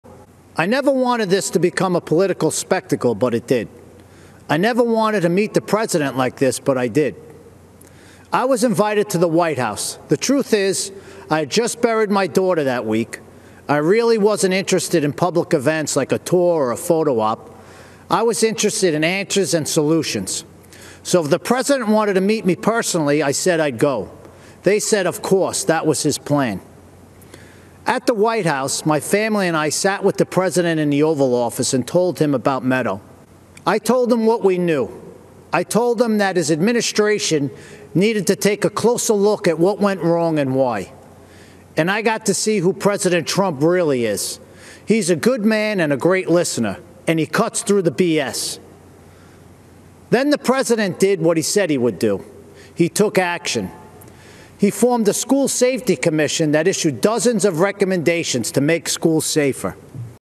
Parkland Victim’s Father President Trump “Took Action” To Make Schools Safer
Andrew Pollack, the father of a Parkland victim, said that President Trump “took action” to make schools safer, during the Republican National Convention on 8/24/2020.